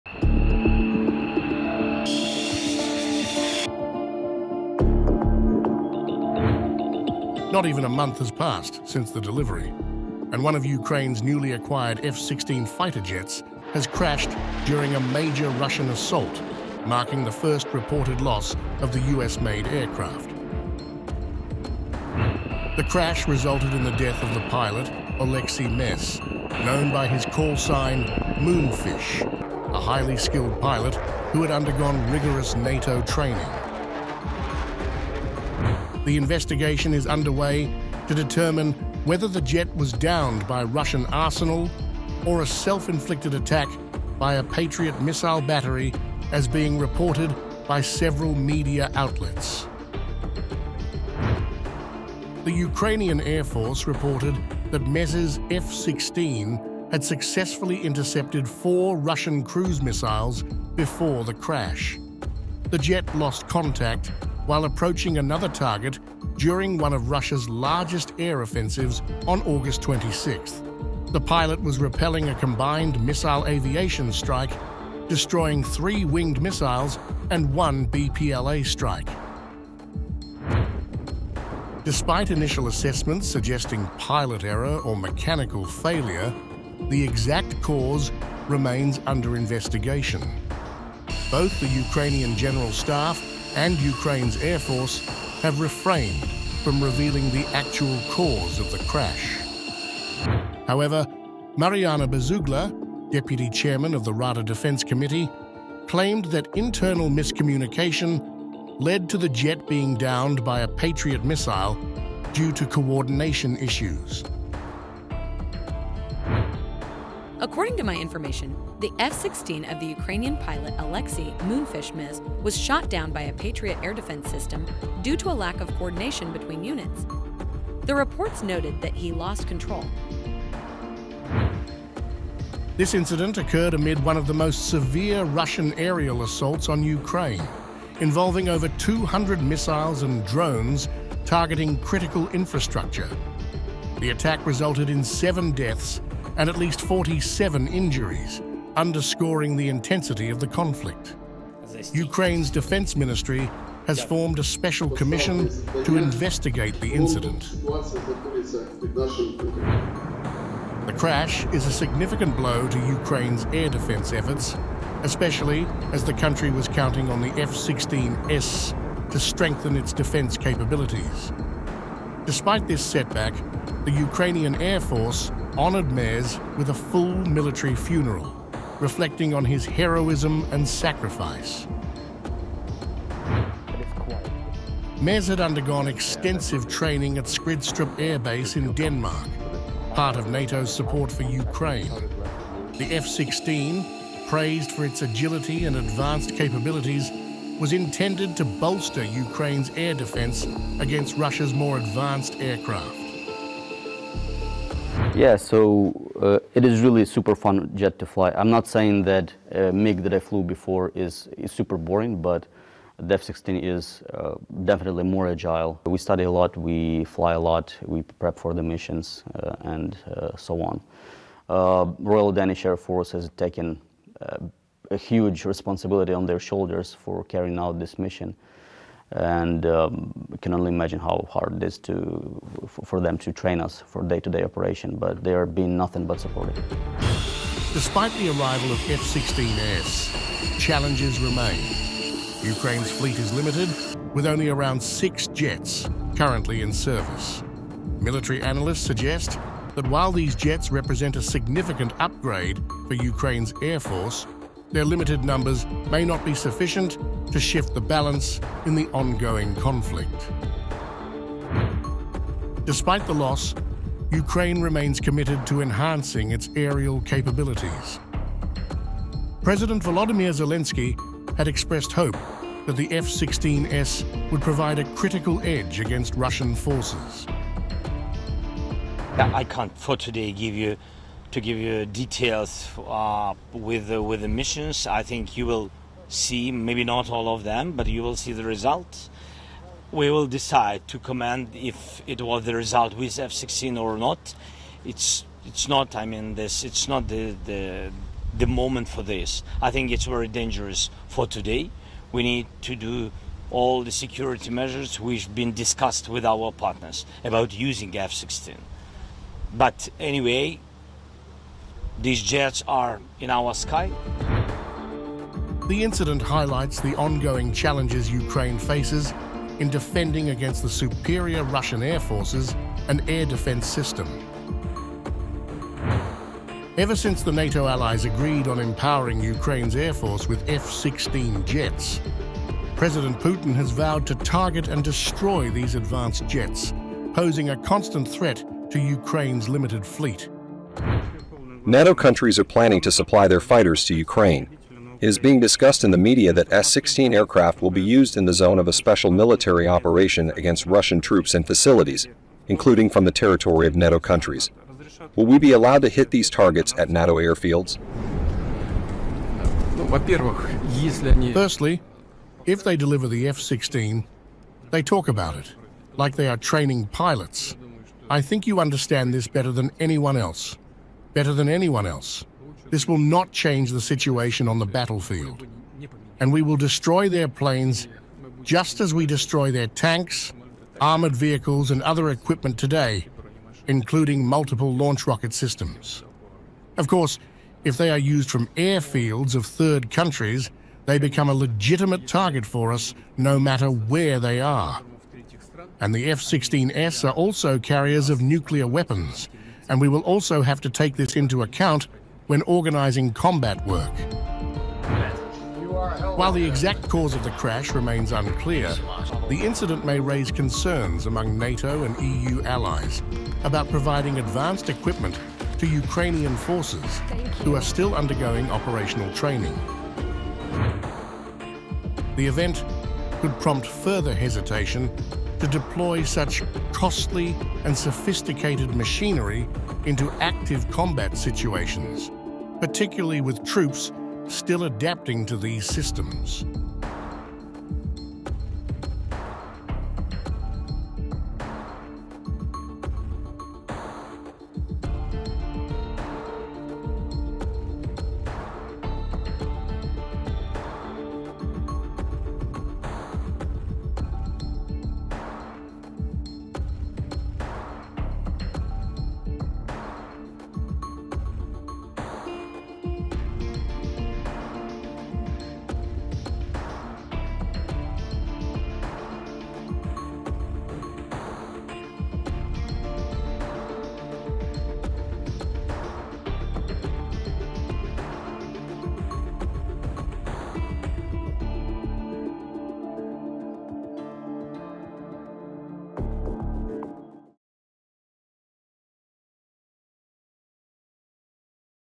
Ukraine's First F-16 Crash: Pilot Killed During Major Russian Assault | Times Now World
Less than a month after receiving them, one of Ukraine's newly acquired F-16 fighter jets crashed during a major Russian assault, marking the first reported loss of this US-made aircraft. Pilot Oleksiy Mes, known as "Moonfish," who had undergone extensive NATO training, perished in the crash. While initial reports suggest potential pilot error or mechanical failure, Ukrainian officials are investigating whether the jet was mistakenly downed by a Patriot missile due to coordination issues. The incident underscores the ongoing challenges Ukraine faces in bolstering its air defenses amid heightened Russian aggression.